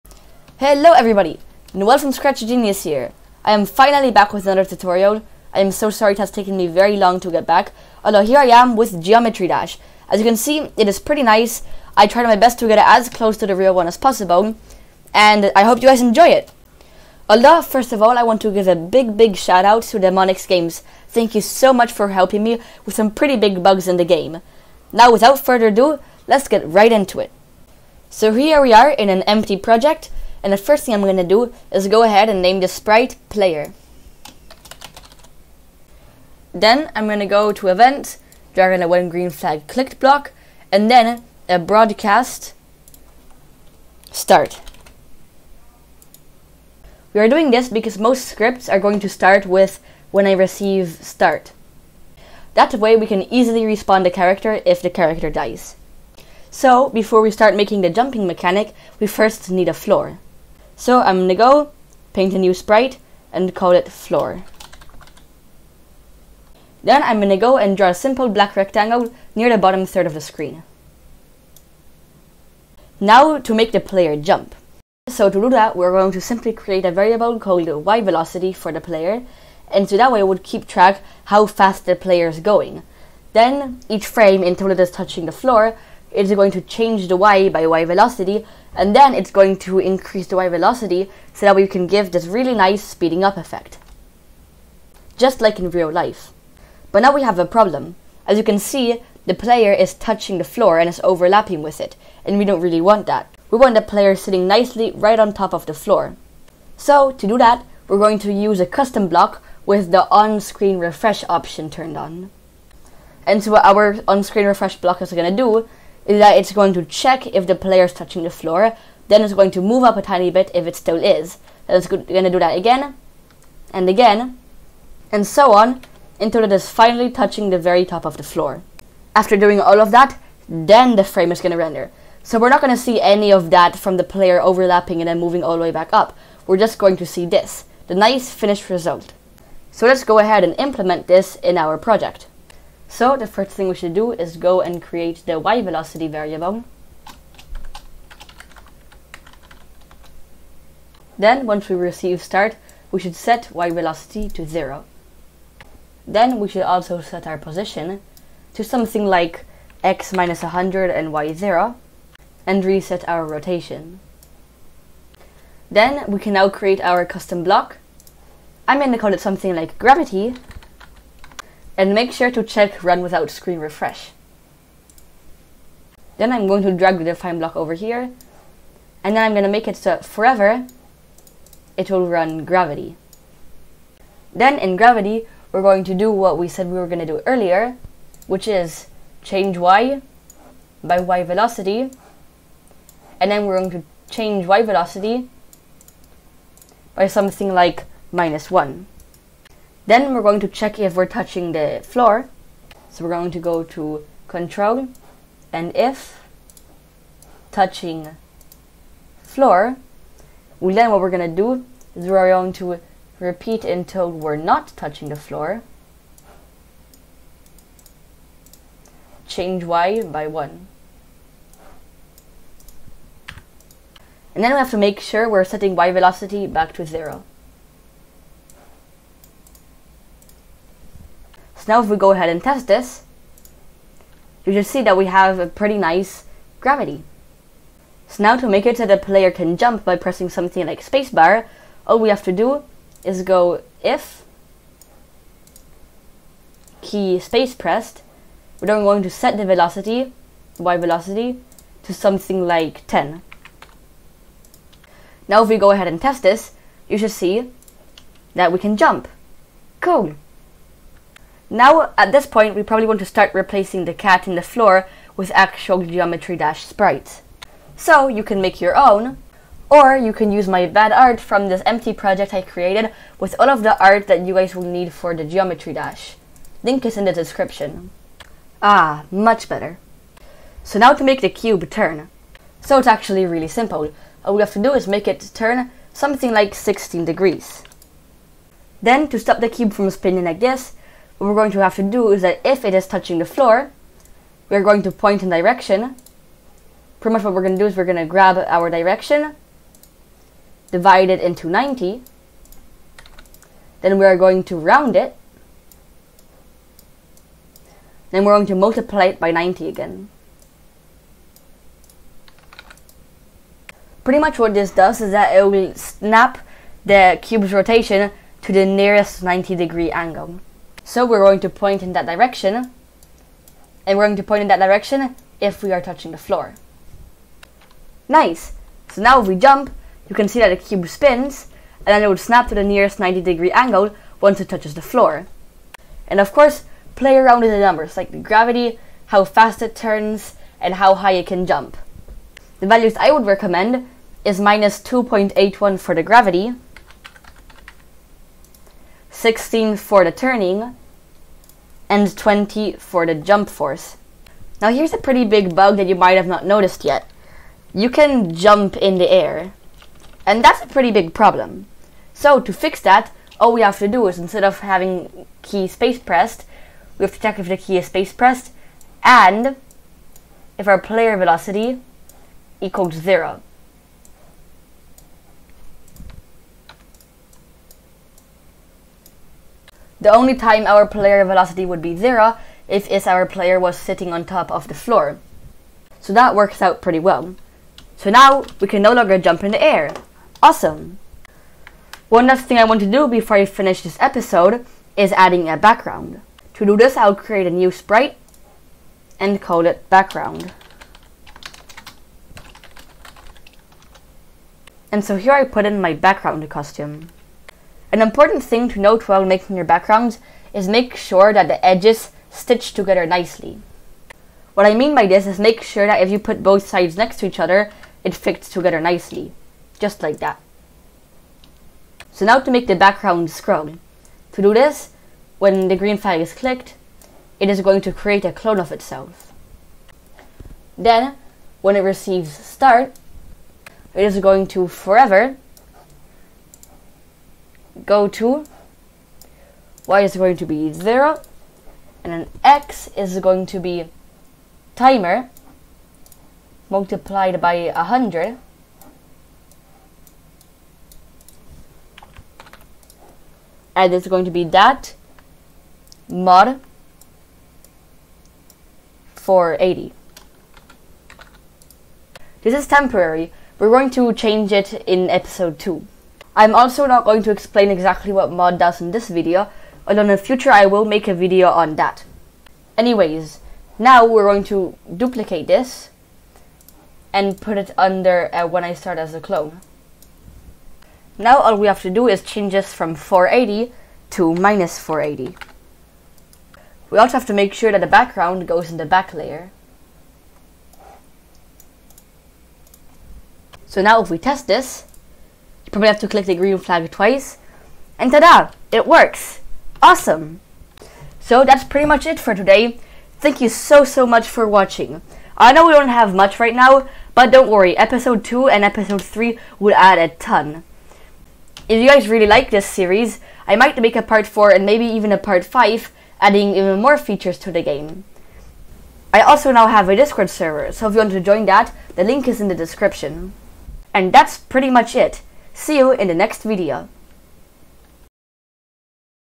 Scratch Geometry Dash tutorial! [Part 1]
Hello everyone!
In this video I am going to be showing you how to make Geometry Dash in Scratch!
This video is meant for beginner to intermediate Scratchers.